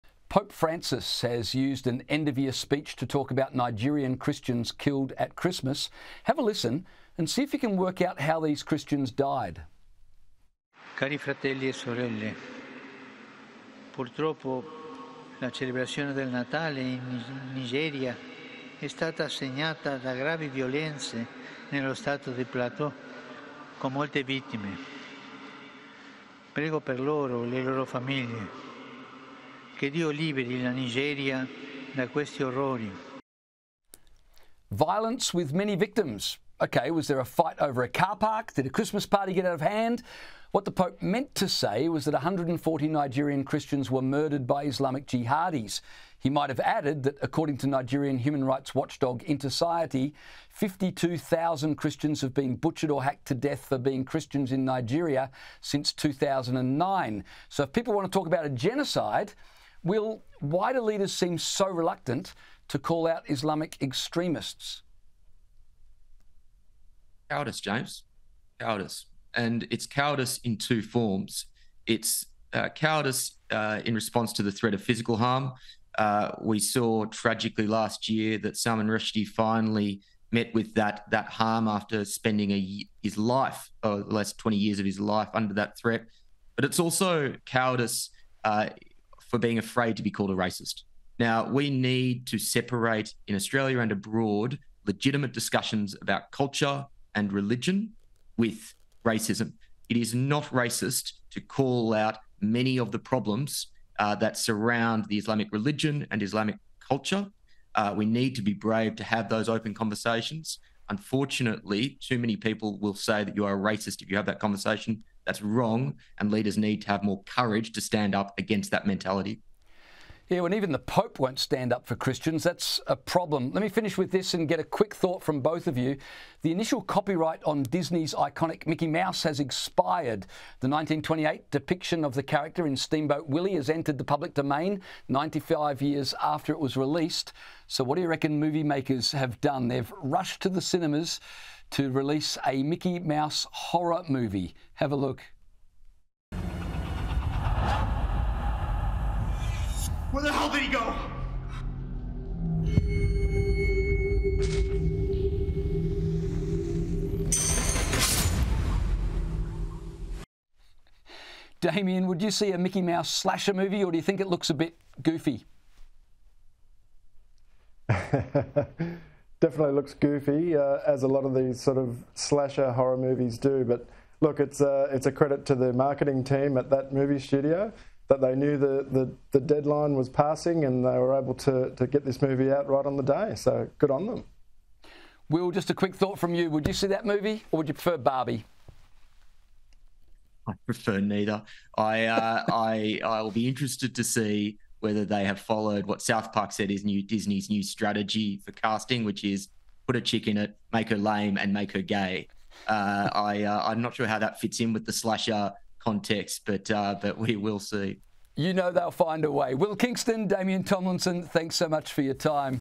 Mickey Mouse horror movie released on day copyright expired a ‘credit’ to the marketing team
The Courier Mail’s Damien Tomlinson says Movie makers are rushing a Mickey Mouse horror movie to cinemas is a “credit” to the marketing team as they were able to get the movie out on the day the copyright expired.

The initial copyright on Disney’s iconic Mickey Mouse has expired.

The 1928 depiction of the character in Steamboat Willie entered the public domain 95 years after it was released.

“Definitely looks goofy, as a lot of these slasher horror movies do,” Mr Tomlinson told Sky News host James Macpherson.

“It’s a credit to the marketing team at that movie studio that they knew the deadline was passing, and they were able to get the movie out right on the day.”